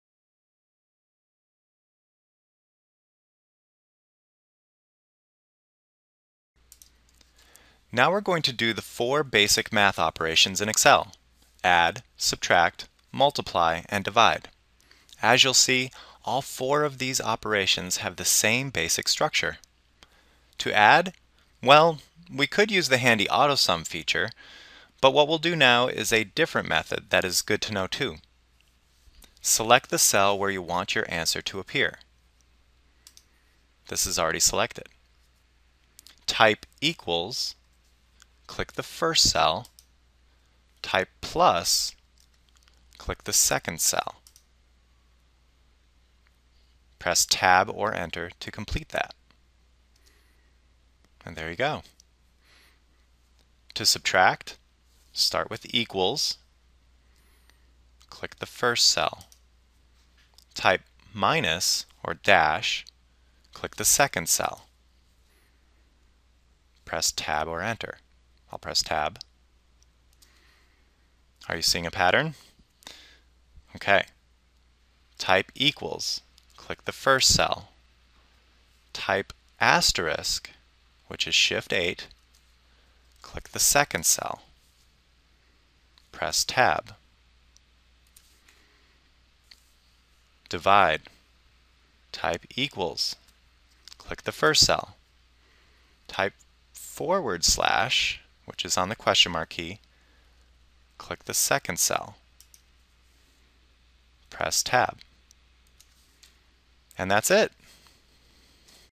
Excel 2010 - Add, subtract, multiply, and divide
This video by In A Minute Learning shows you how to add, subtract, multiply, and divide numbers in Excel 2010.